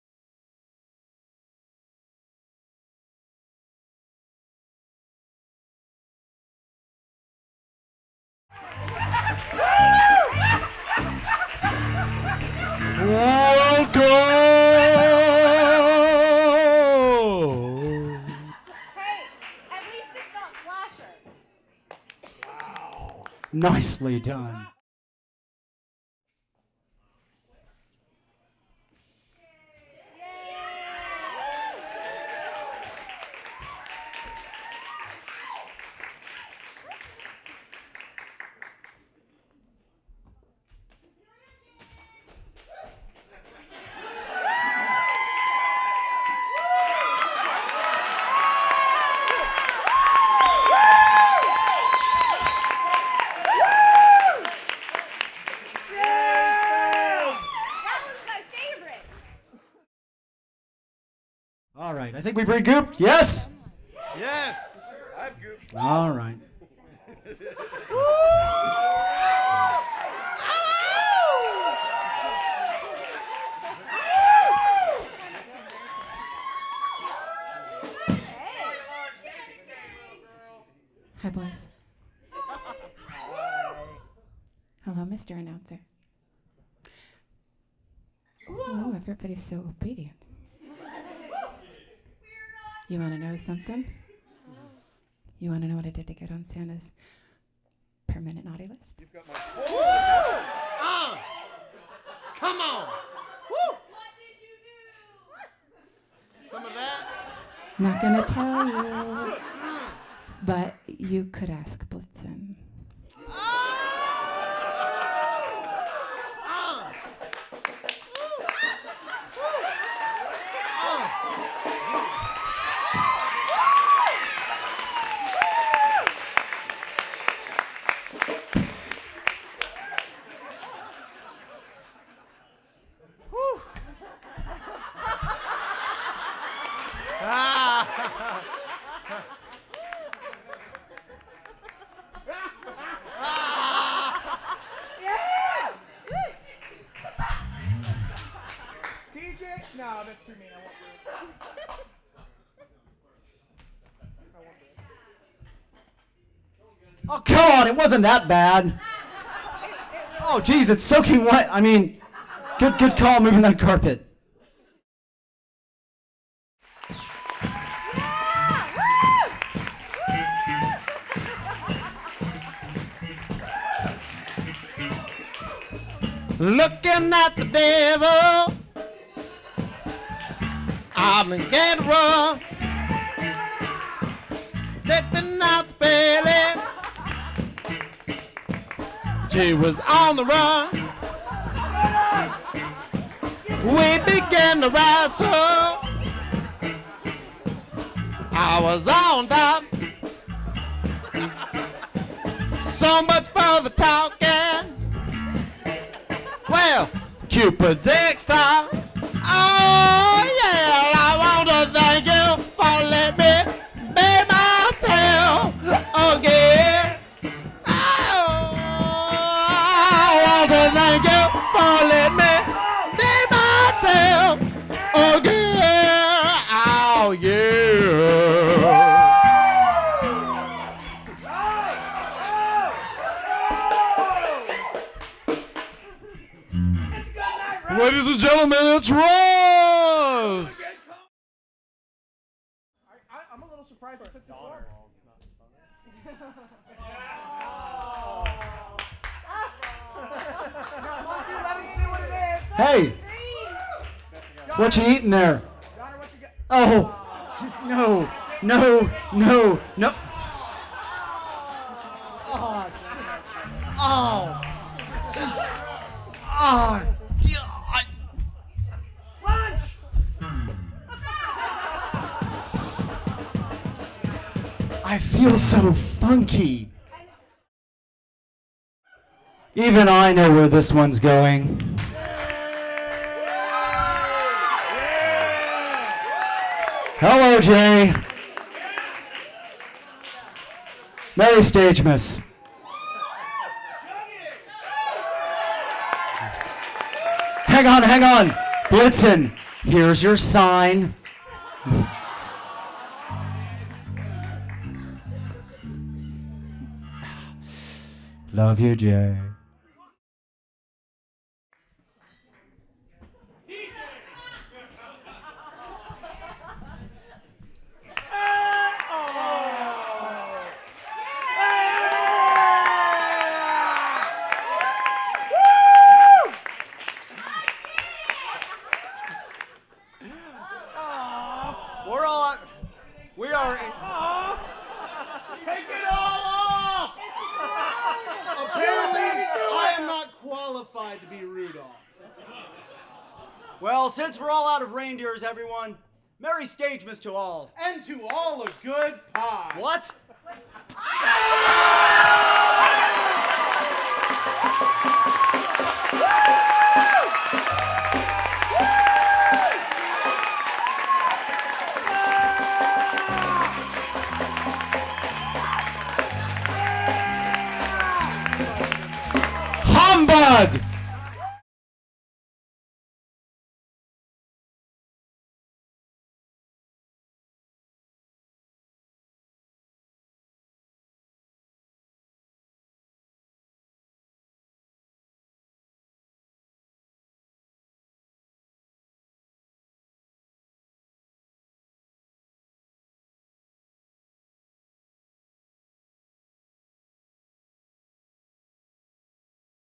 Open Stage Extras 12/19/11 - Santa's Little "Helpers"
A compilation of the reindeer gags from our December 19th show.

The Reindeer:

Attention Deficit Dasher - Garrett "Li'l G" Runnels
Dancer - Rachel Hullett
Fabulous Prancer - Philip "Fez" Rodriguez
Vixen - Kelly Nygren
Cleanup Comet - TJ Whiskernibbles
Kissing Cupid - Kasha Reese
Donner (Party) - Garrett "Monkey" Jacks
Blitzen - Jay Redden
Rudolph - John "Valos" Lowe

Open Stagemas - 12/19/11
Performer: